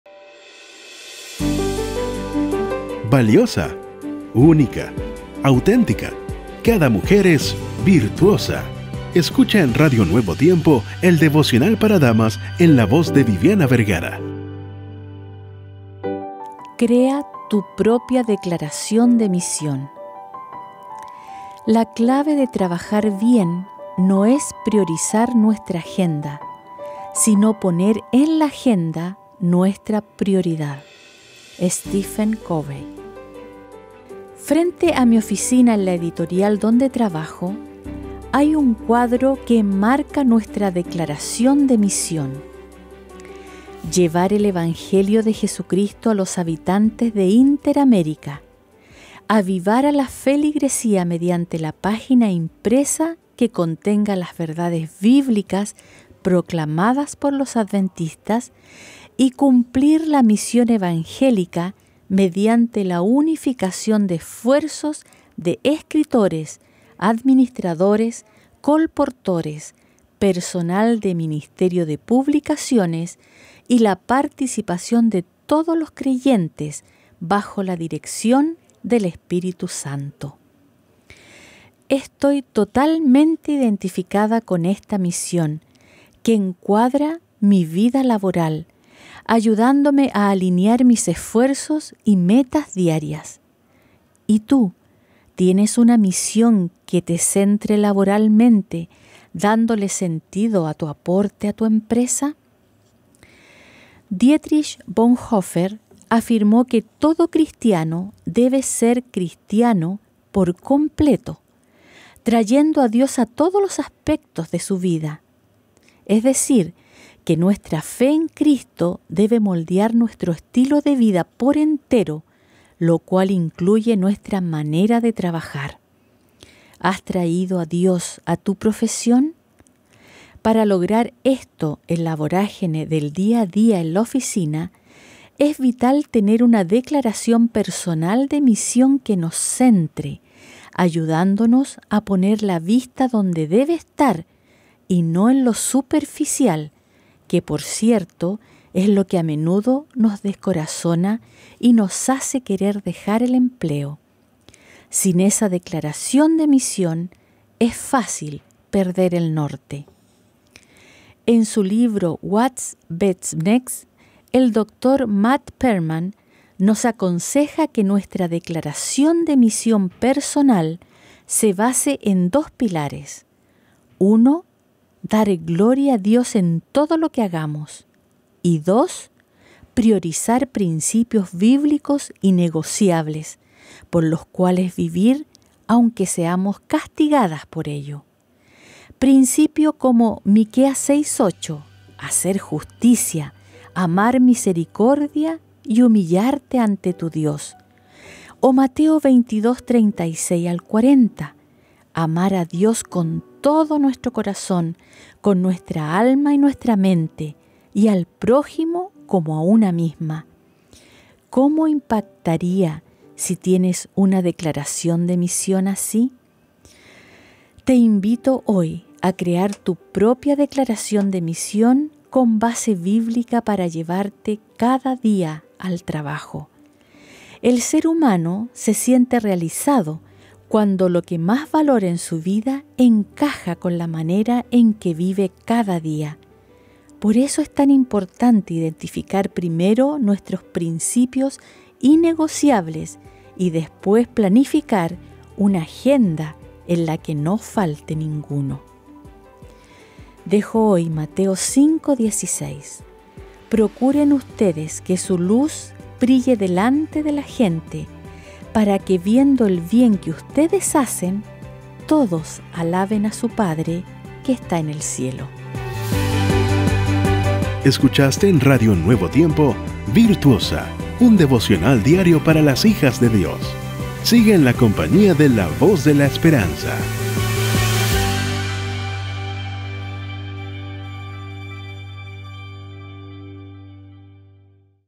Devoción Matutina DAMAS 8 de noviembre 2024 - "Crea tu propia declaración de misión" | Virtuosa
💐Recuerda siempre que al ser luz para los demás, no solo glorificas al Padre celestial, sino que también siembras pequeñas semillas de amor que pueden crecer y cambiar vidas.✨😊

📖 Lee para hoy: Mateo 5:16